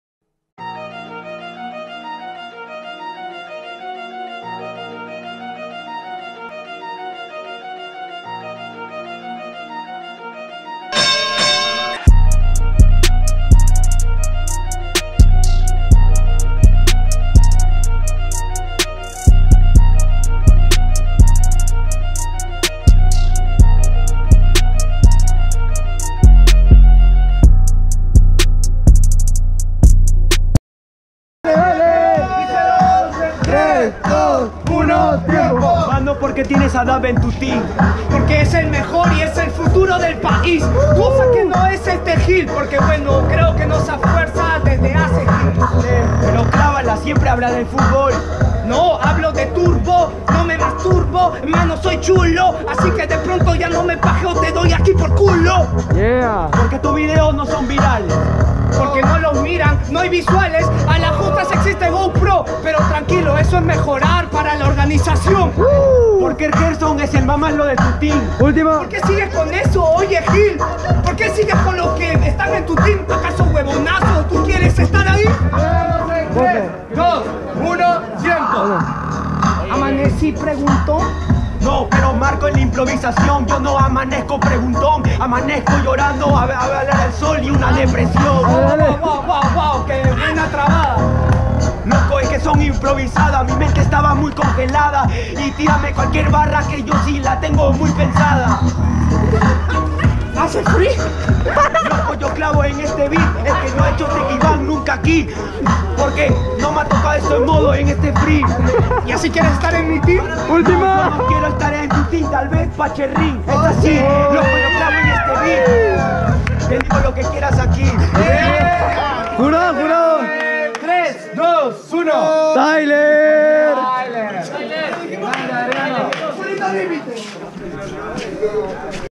FUCKING TYLER VS MC - CUARTOS DE FINAL - MAGDALERAP - AUDICIÓN PACHE RING REGIONAL
Ya disponible la Audición a la Regional Pache Ring de Magdalerap